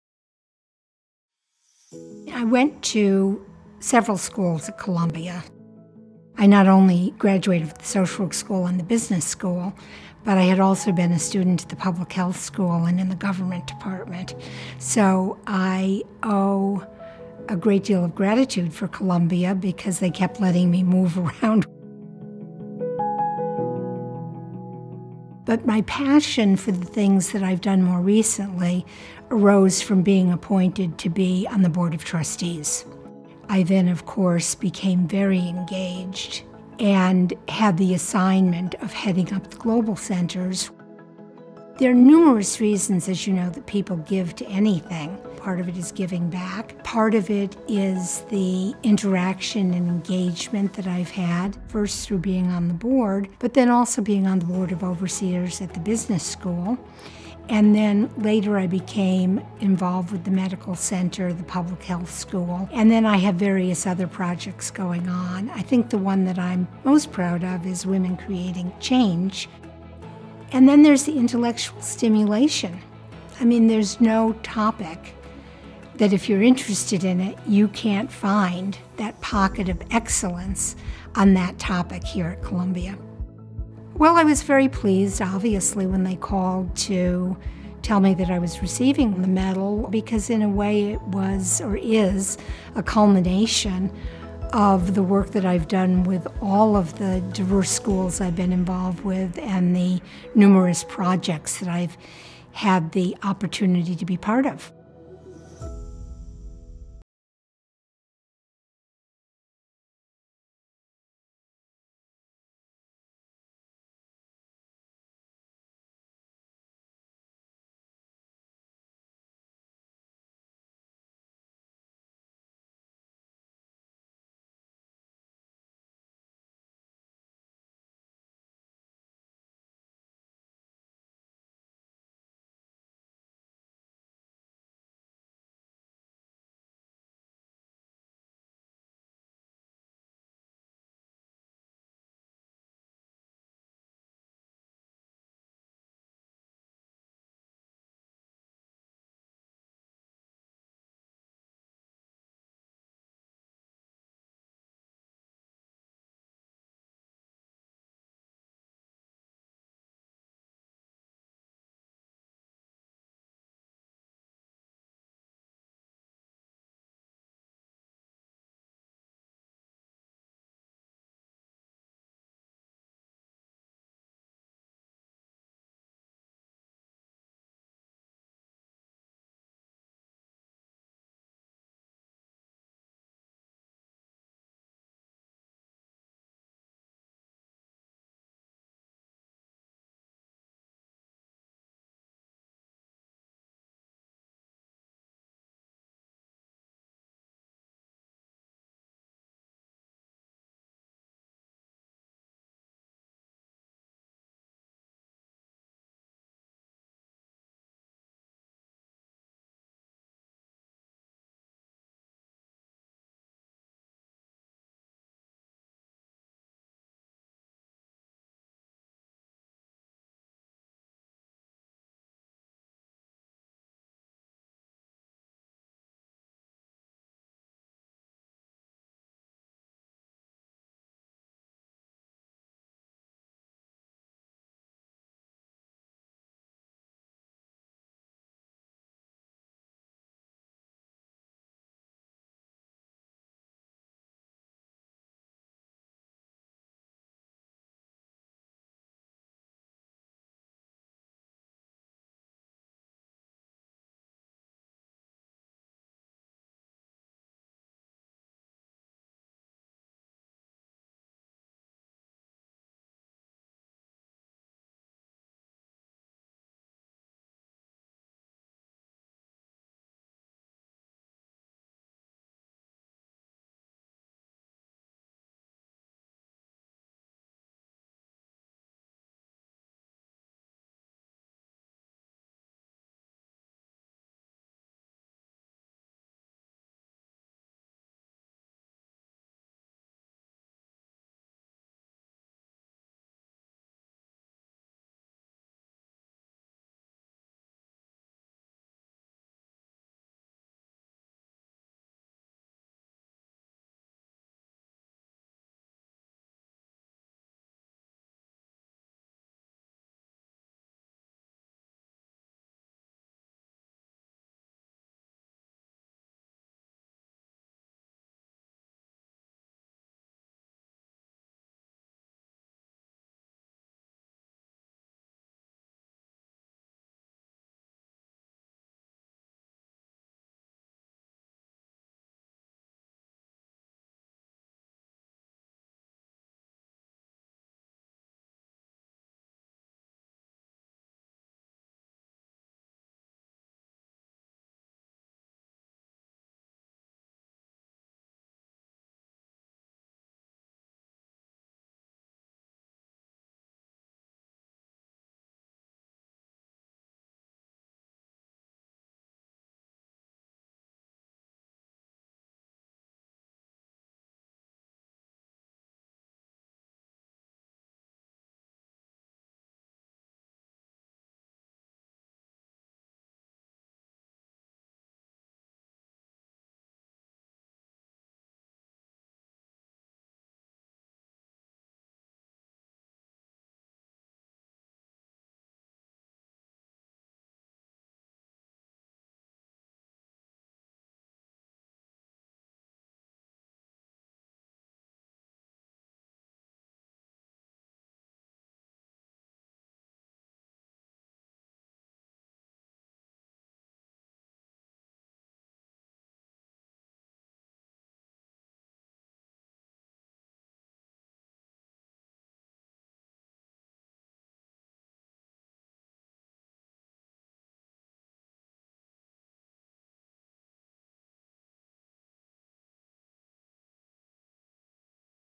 Ann Kaplan '72SW, '77BUS – 2022 Alumni Medalists
Ann F. Kaplan ’72SW, ’77BUS is a graduate of the Columbia School of Social Work and Columbia Business School. She is a Columbia University Trustee Emerita and, as the Chair of the Committee on Global Initiatives and Co-Chair of the Global Leadership Concil, had the pleasure of supporting the establishment of the network of Columbia Global Centers. She also served on the board of the Columbia University Investment Management Company and, as Vice Chair of the Finance Committee, chaired the Committee on Social Responsibility. Kaplan is currently a member of the Board of Overseers of Columbia Business School where she serves on the Governance and Nominating Committee, is a Founding member of the Women’s Circle, has supported the Manhattanville Campus, and has endowed a chair. She also serves on the Board of Advisors at the Columbia University Irving Medical Center, the Mailman School of Public Health, and the Beijing and Amman Global Centers. Kaplan is the Chair of the Women Creating Change Leadership Board, which supports a Columbia grant giving program for interdisciplinary work on issues that affect women. She also taught asset management at Columbia Business School from 2004 to 2014. She supports projects at The Global Centers, the Medical Center, and the Center for Chronic Grief.